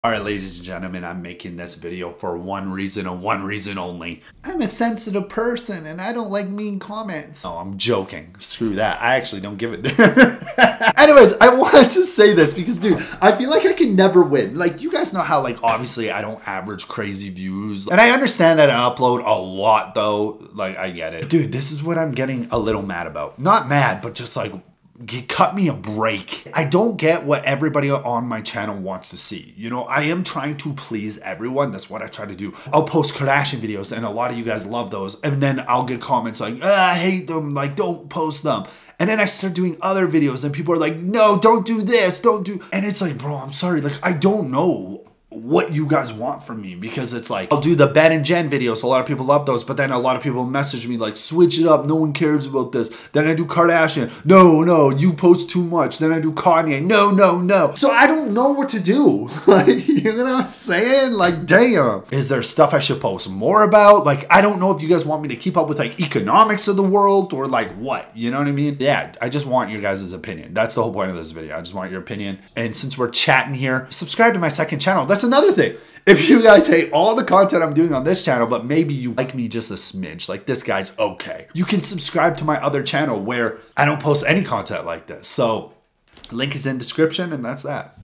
IM SORRY!!!! | What Should I DO!!?!?!
IM SORRY! | WHAT SHOULD I DO!!?!?!!??! | so I upload A LOT of Drama videos and thats no secret but from Kardashian Jenner content to Jennifer Lopez, Ben Affleck, Kanye West and more. I can't please everyone